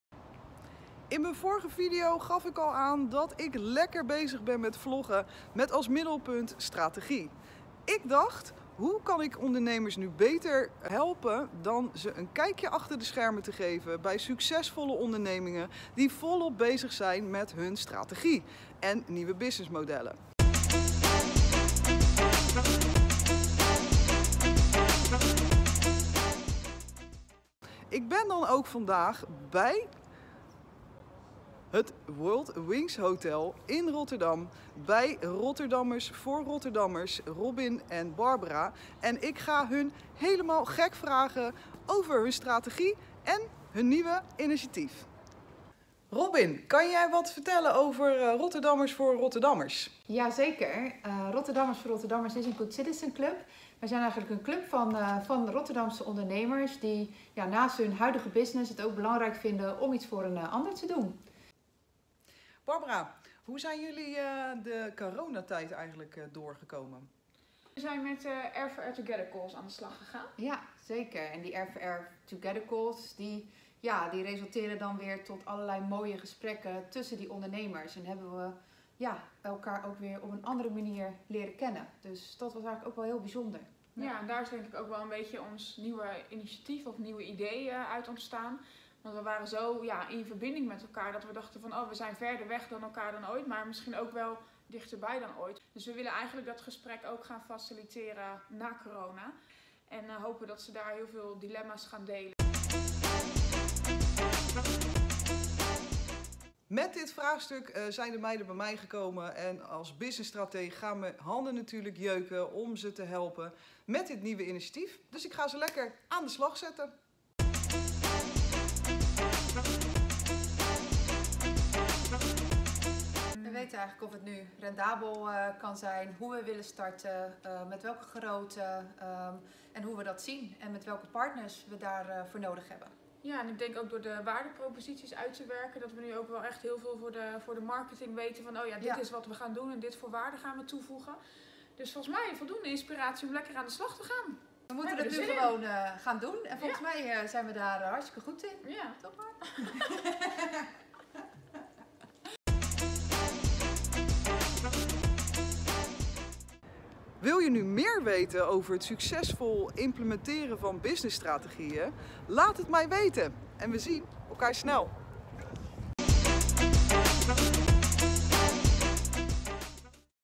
Vlog #2 Strategiecreatie_Rotterdammers4Rotterdammers
Van idee naar strategiecreatie, waarom zou je dat doen?

De dames Robin en Barbara van Rotterdammers 4 Rotterdammers leggen je dit haarfijn uit.
Het creëren van strategie helpt jou als ondernemer bij het vaststellen van een goede format voor jouw nieuwe businessmodel.
Het geeft o.a. antwoord op de vragen: Is mijn idee haalbaar en rendabel? Wie zijn mijn klanten? Welke waarde lever ik aan mijn klanten? Welke mensen en middelen heb ik daarvoor nodig? En uiteindelijk welke stappen je vervolgens moet zetten. Kortom, van strategiecreatie naar realisatie!

Hulp nodig? Laat het mij weten.
Reacties op mijn vlog zijn welkom.

We zien elkaar snel!